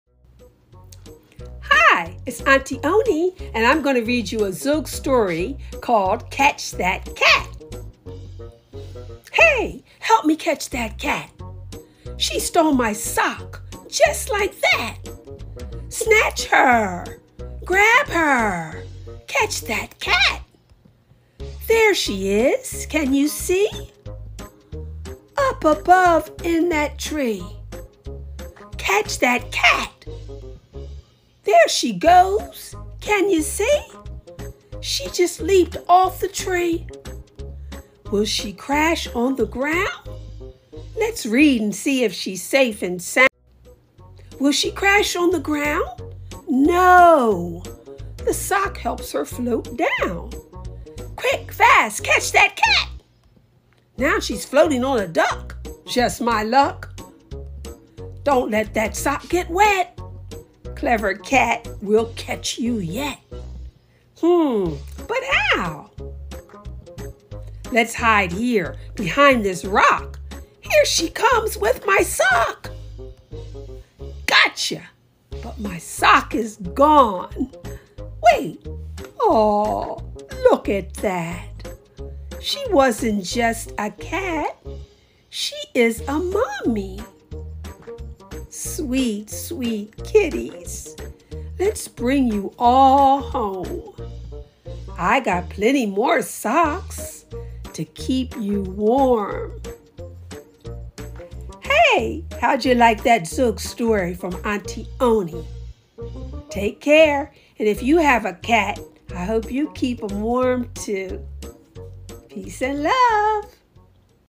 ZOOG Story *!* Catch That Cat ~ (full story)
Aunti Oni tells about a runaway cat!
Everyone can have fun reading stories, singing songs & cracking jokes.